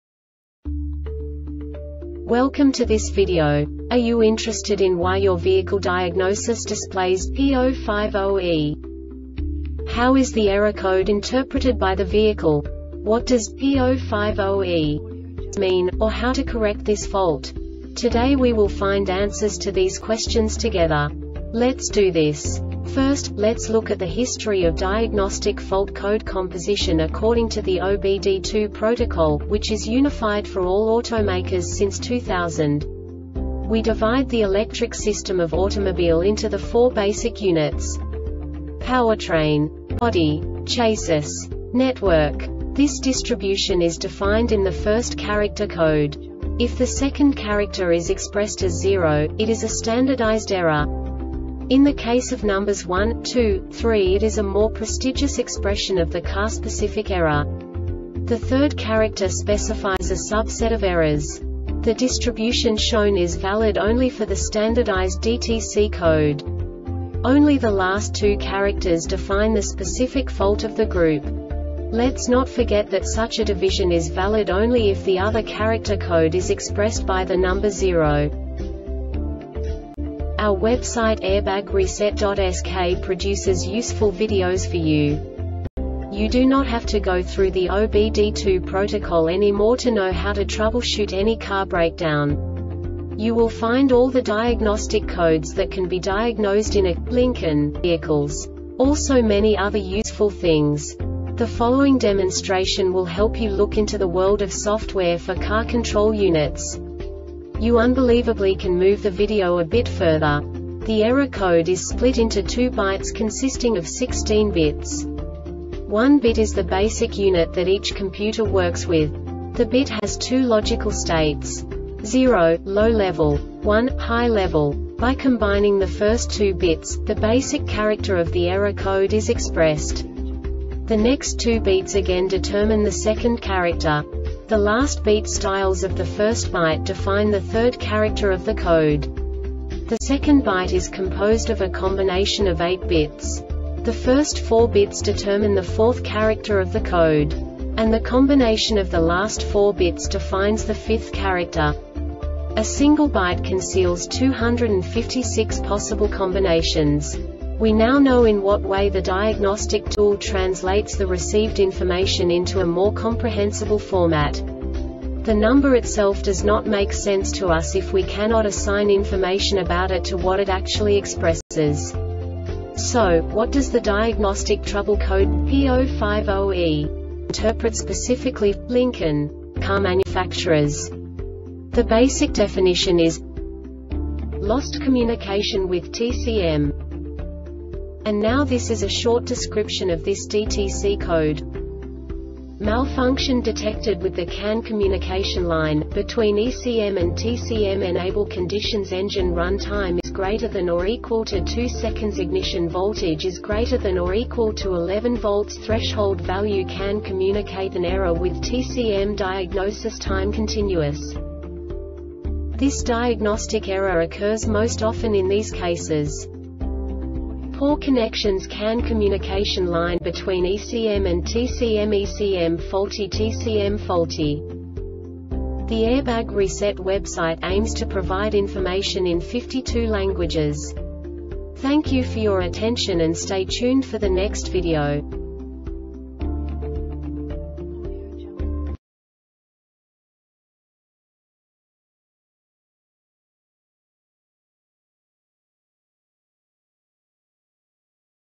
DTC Lincoln P050E Short Explanation
Contents:
0:21 Basic DTC analysis according to OBD2 protocol standard.
1:48 Insight into programming
2:58 A diagnostically understandable description of the Diagnostic Trouble Code
3:05 Basic error definition

Make:
Lincoln
Code:
P050E
Definition:
Lost Communication With TCM
Description: